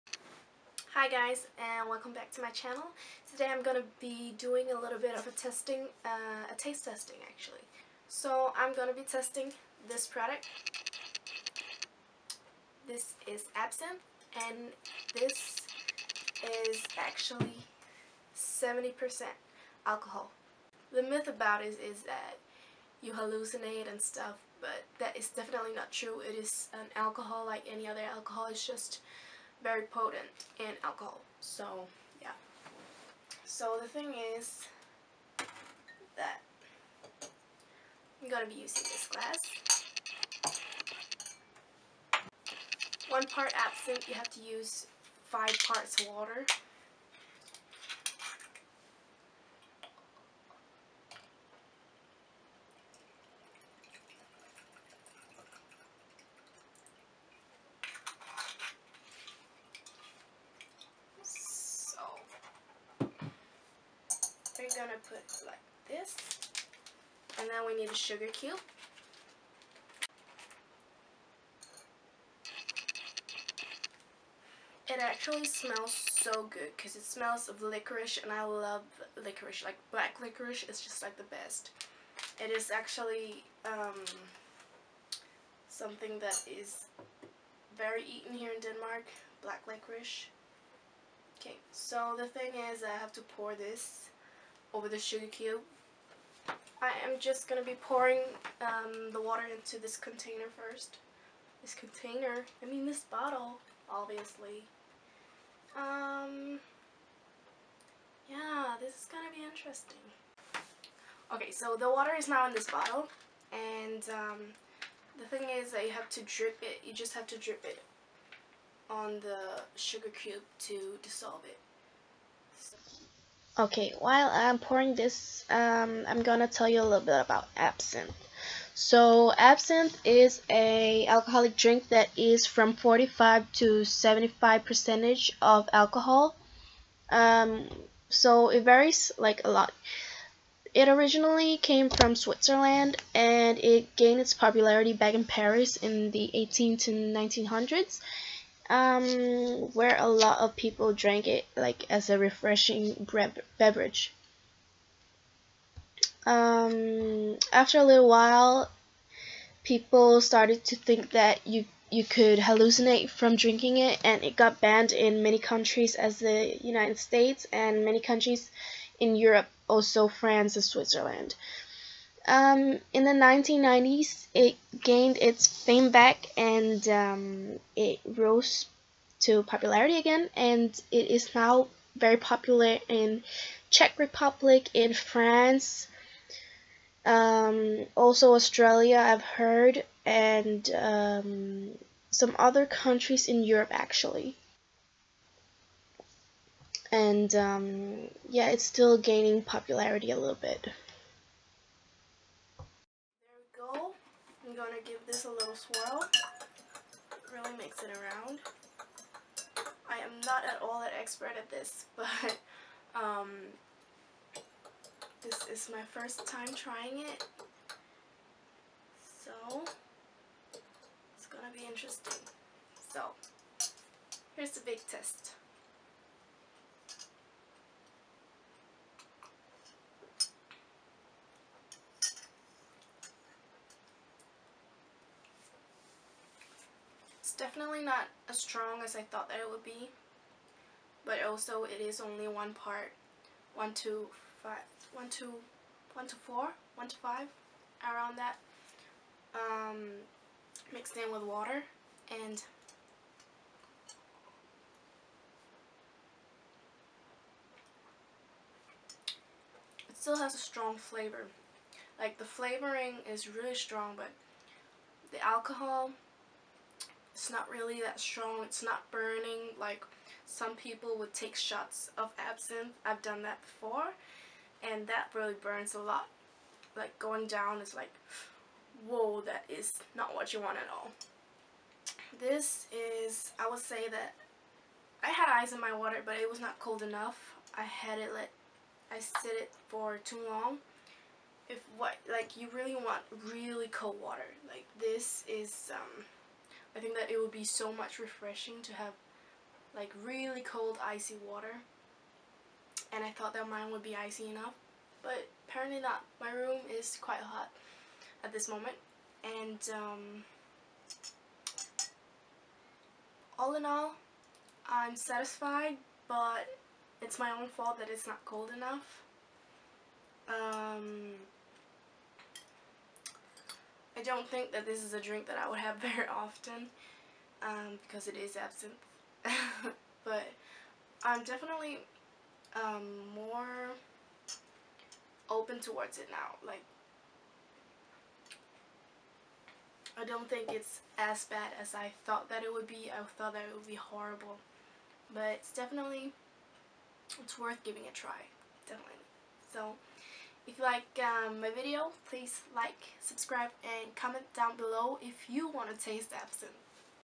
TASTE TESTING: ABSINTHE
Today i got a bottle of Absinthe and i decided to share my taste testing with you guys.
It really taste extraordinary and i would recommend anyone to try to at least taste it because it is a taste like nothing else.

Camera im using is a Nikon D3100